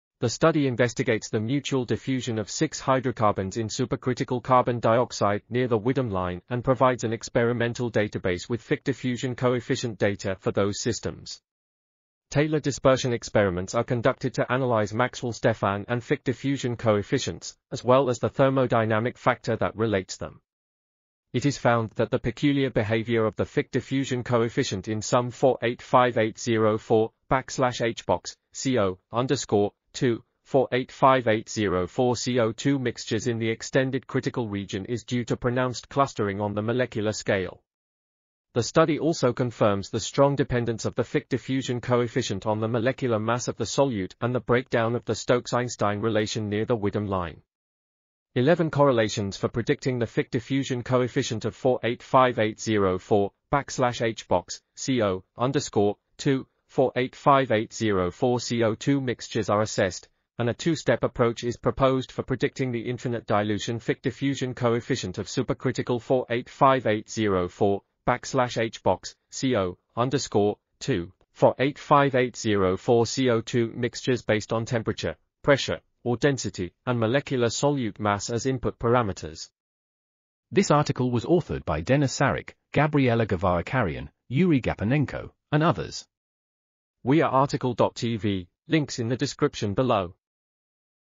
Diffusion of hydrocarbons diluted in supercritical carbon dioxide | RTCL.TV
### Keywords ###

### Article Attribution ###
Title: Diffusion of hydrocarbons diluted in supercritical carbon dioxide
Authors: Denis Saric, Gabriela Guevara-Carrion, Yury Gaponenko, Valentina Shevtsova ,and Jadran Vrabec

### Video Timestamps ###
0:00:00 - Summary
0:01:20 - Title
0:01:26 - Tail
0:01:30 - End